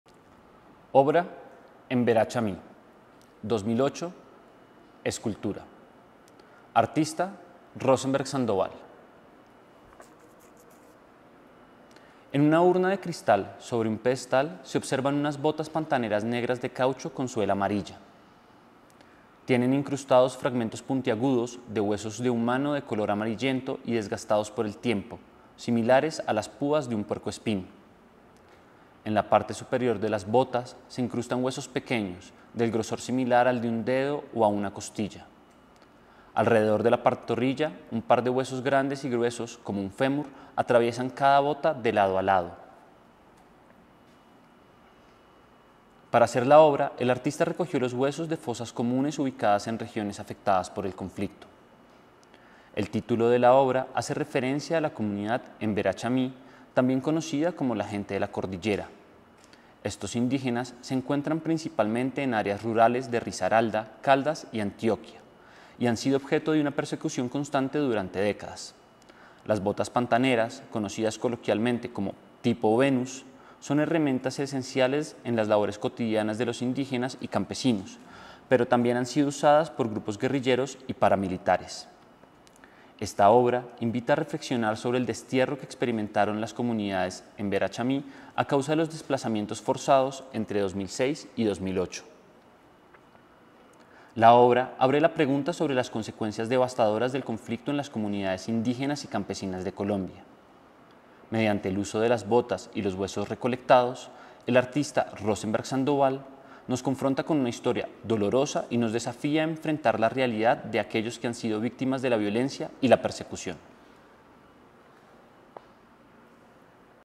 9. Circuito inclusivo MAMBO: obra “Embera Chami” Rosemberg Sandoval
Video audiodescriptivo con intérprete en lengua de señas para personas diversas sobre la obra “Embera Chami” escultura, 2008. Esta obra abre la pregunta sobre las consecuencias devastadoras del conflicto en las comunidades indígenas y campesinas de Colombia.